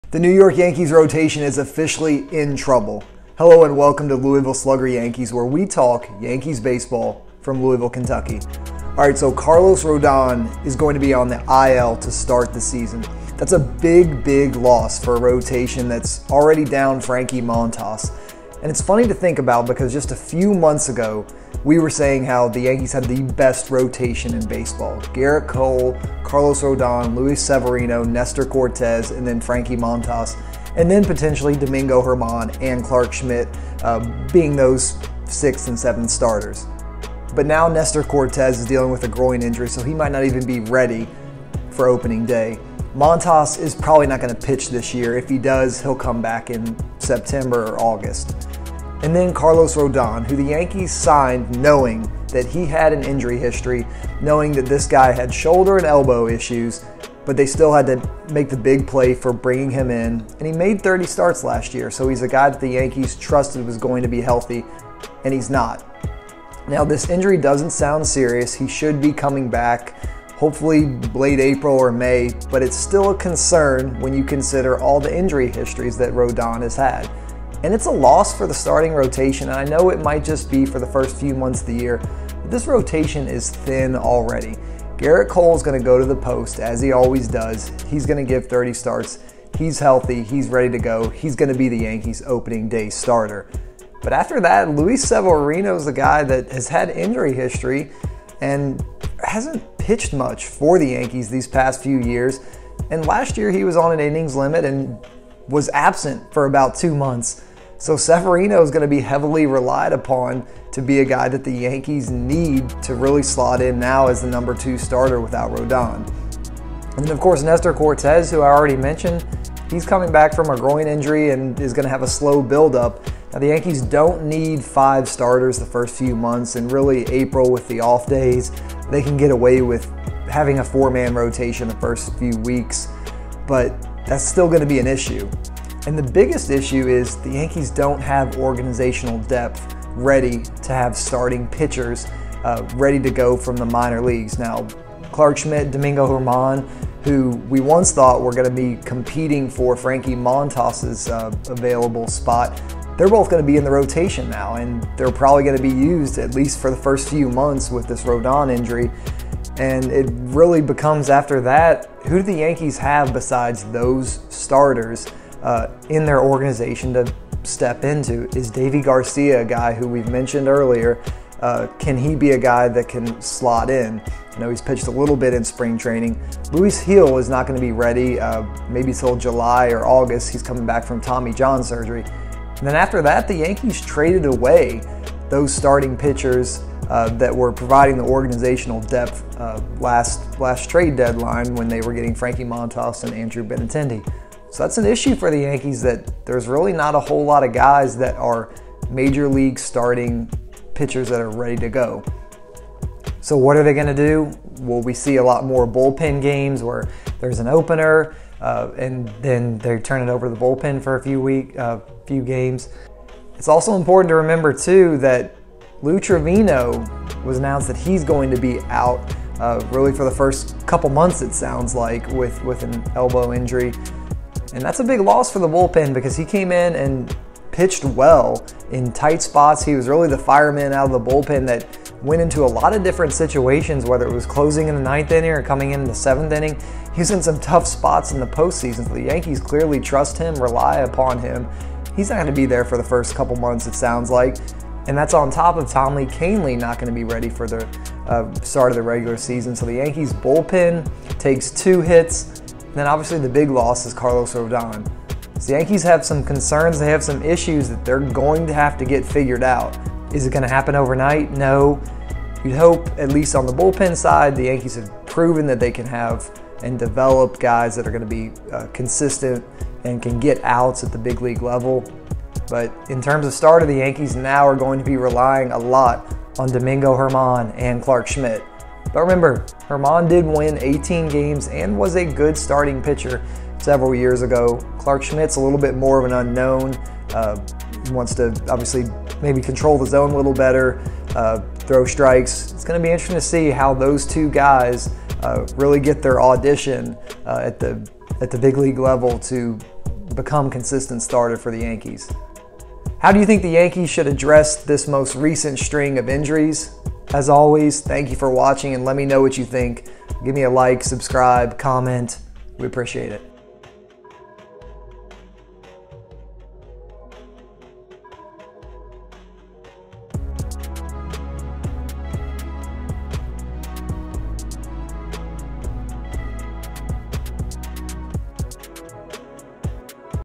New York Yankees rotation 'in trouble' after Carlos Rodon injury
Carlos Rodon will start the season on the IL with a forearm injury, announced on March 9. The New York Yankees signed the starting pitcher to a $162 million contract this offseason.

Rodon was preparing to be the Yankees' No. 2 starter behind Gerrit Cole, followed by Luis Severino and Nestor Cortes, who is recovering from a groin injury.

The starting rotation has already been hit with injuries this offseason. Frankie Montas isn't expected to return until late in the season after shoulder surgery, while Cortes won't be ready for Opening Day.

Lou Trivino and Tommy Kahnle will also start the season on the IL, two losses to the Yankees' bullpen depth.

Clarke Schmidt and Domingo German are expected to slot into the rotation.